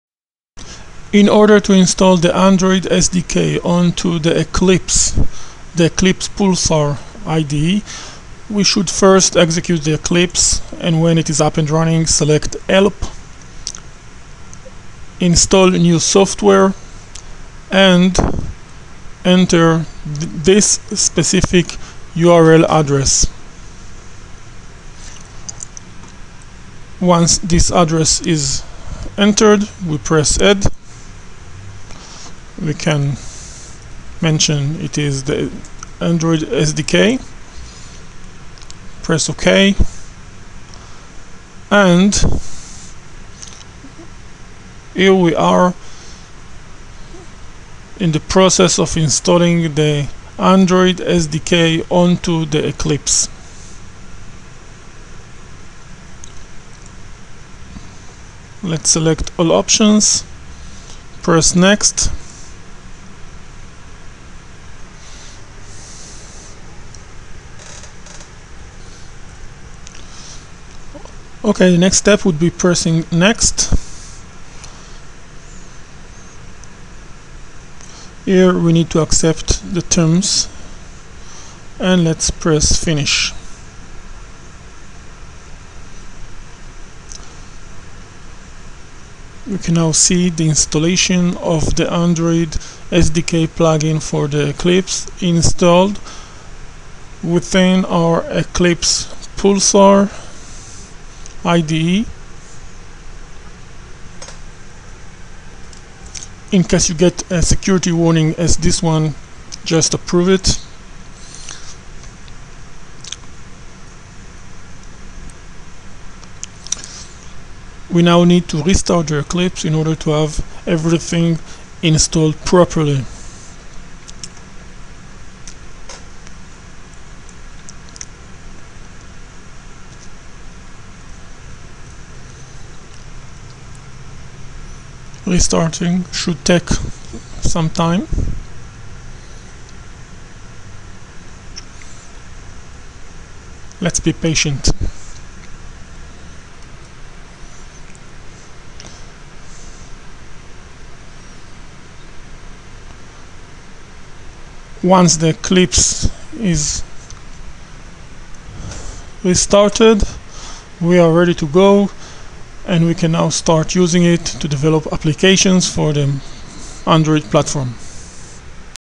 Installing Android SDK Plugin for Eclipse IDE
Short video clip that explains how to install the Android SDK plugin for the Eclipse IDE. When following the guidelines make sure you enter the correct URL shown at 0:22 timeline

This is one video out of five videos that explain in five steps how to set up the required environment. More info about these five steps can be found More info about the course can be found

Good Luck!
Haim.